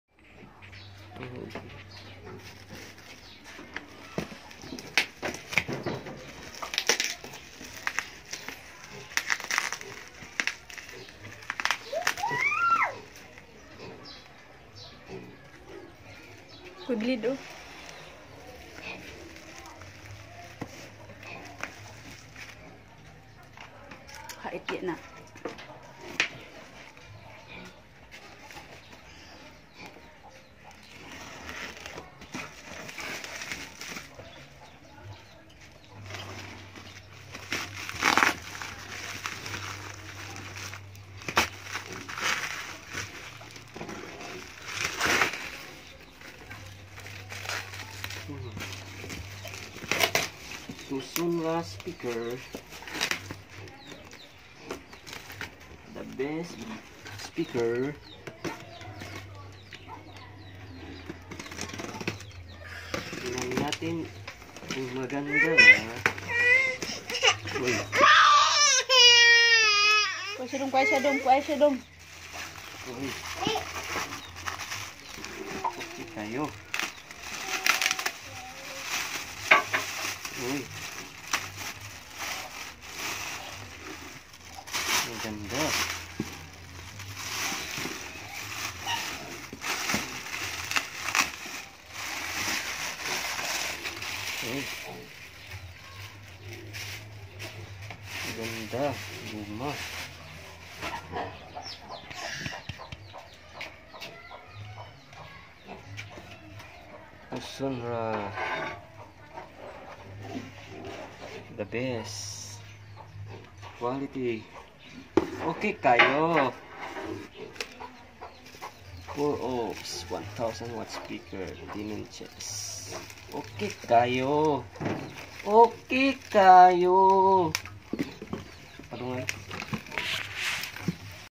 Tosunra Subwoofer speaker 10" 1000wats unboxing
tosunra speaker
1000wats
4ohms
10inches
rubercone stainless
subwoofer s

nabili ko to sa lazada sa halagang 790 pesos at may shipping fee na 90pesos
maganda pulido buo ang bass buo ang tunog pangmatagalan original